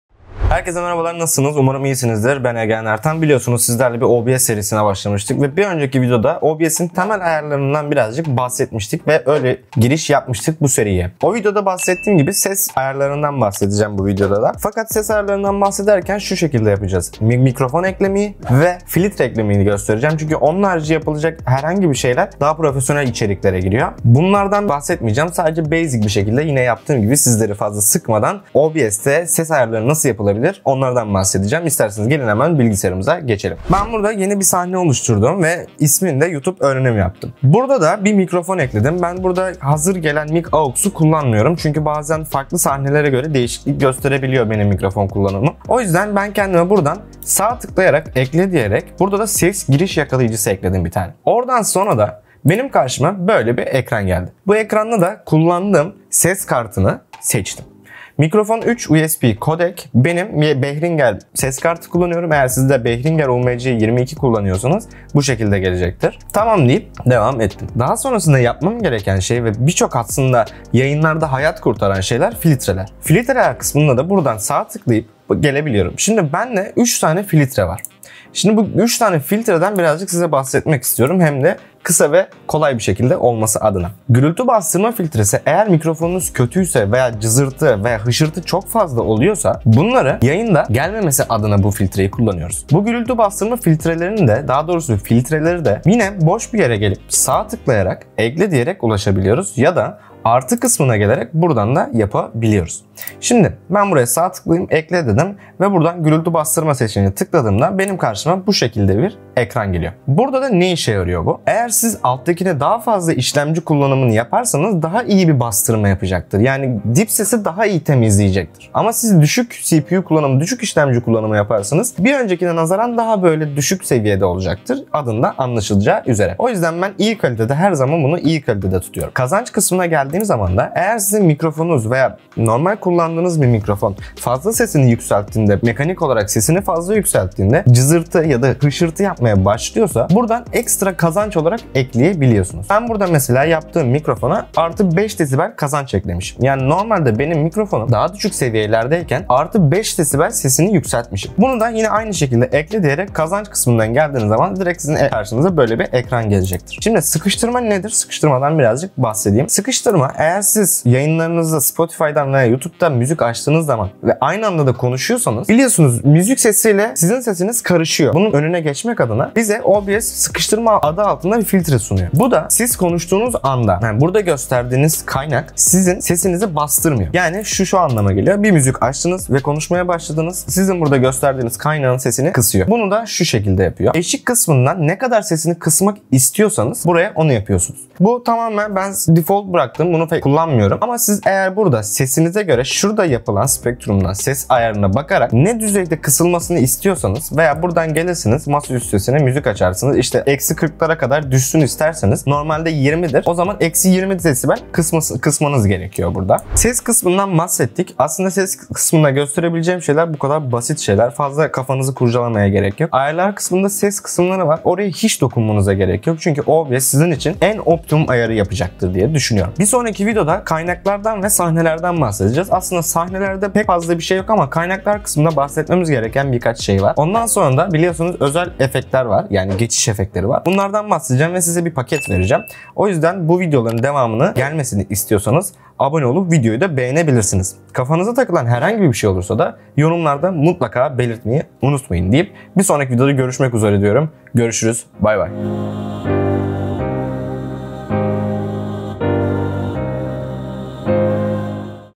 KALİTELİ SES SIRRI - Obs SES AYARLARI ve SES EFEKTLERİ Ayarları #2
Herkese merhabalar bu videoda obs ses ayarları nasıl yapılır ondan bahsettim. Yayınlarınız da veya aldığınız kayıtlarda ses kalitenizi bir adım daha öne taşıyabilecek ses ayarlarından bahsettim. Bu ayarlar ile profesyonel obs ses ayarına kavuşmuş olacaksınız. Ses ayarlarının yanında ise obs ses efektlerinden bahsettik. Obs ses efektleri nasıl kullanılır diye soruyorsanız kesinlikle bu videoda cevabını bulacaksınız.